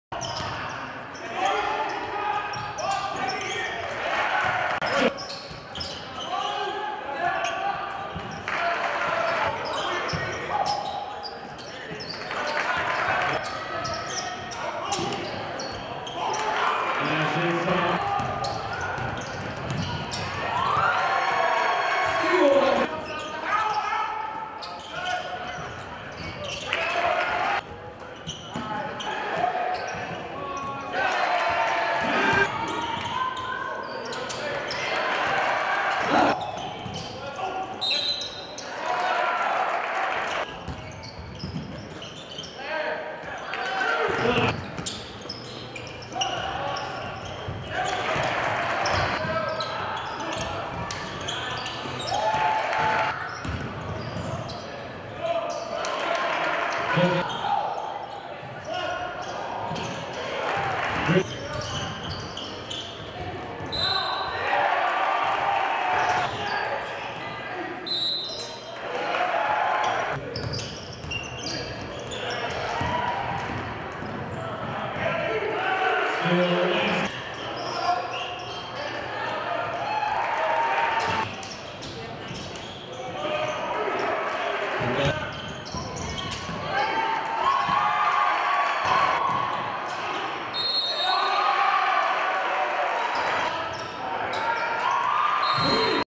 Gardner-Webb Men's Basketball: Highlights vs. Brevard (11-29-14)
Check out highlights from Gardner-Webb men's basketball's 78-63 win over Brevard College earlier today.